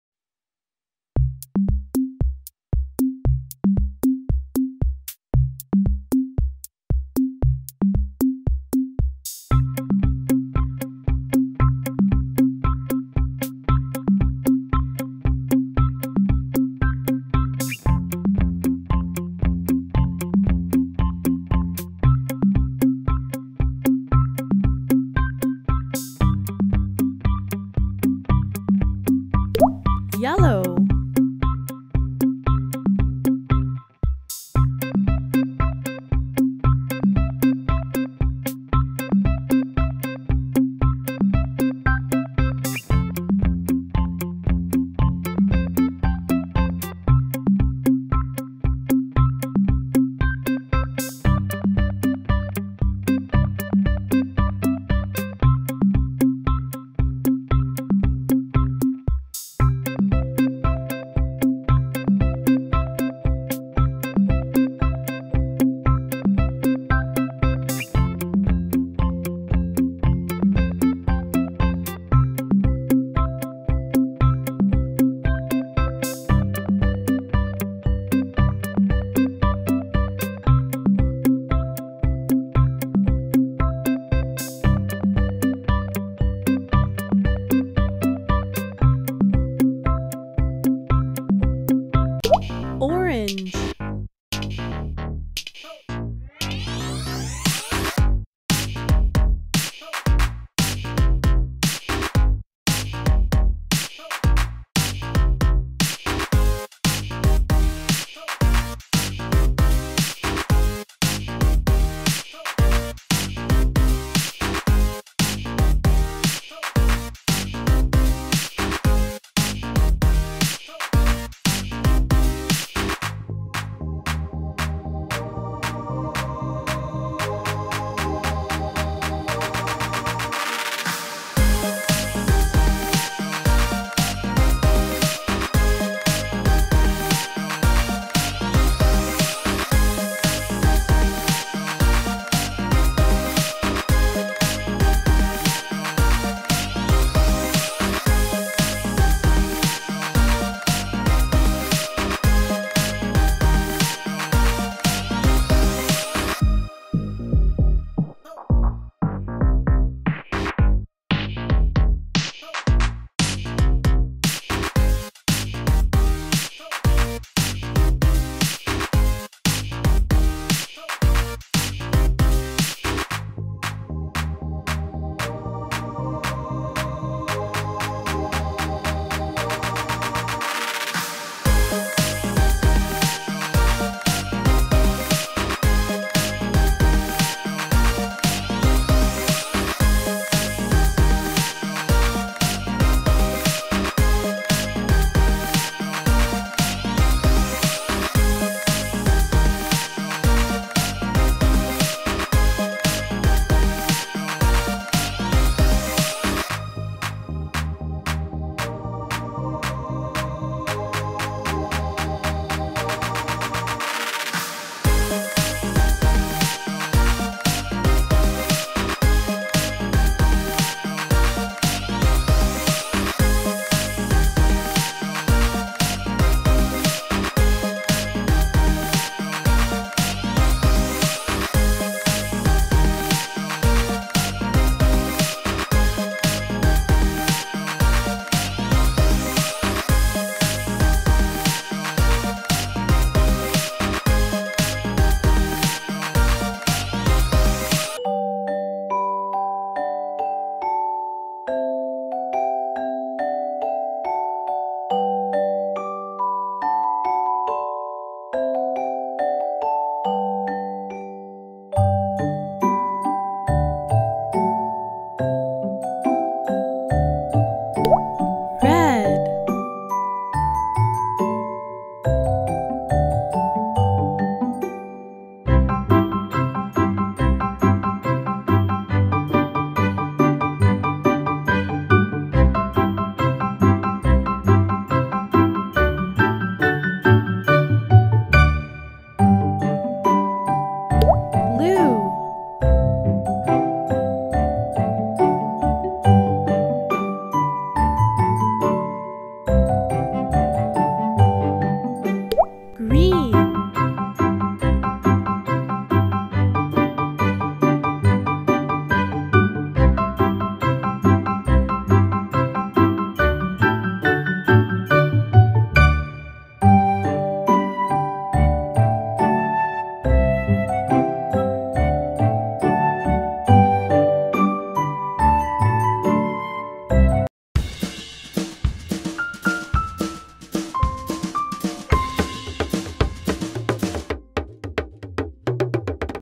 💜Coloring a Toy Clock Painting Pages for Baby, Learn Colors and Coloring
I'm Coloring a Toy Clock Painting Pages for Baby with Paint.  Learn Colors for Kids and how to color a toy clock!